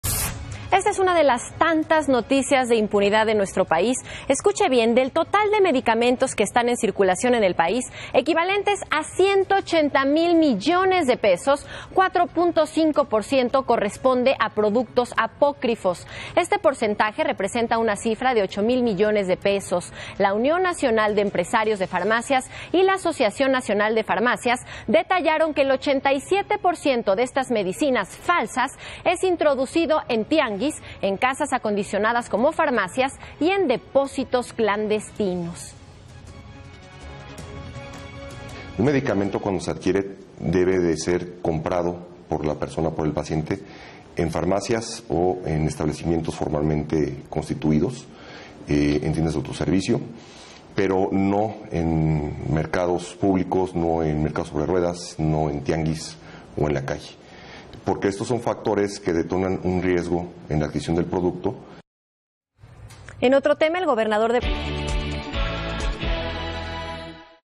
4.5% de los medicamentos son apócrifos
El 87% de estos es introducido en tianguis o mercados callejeros.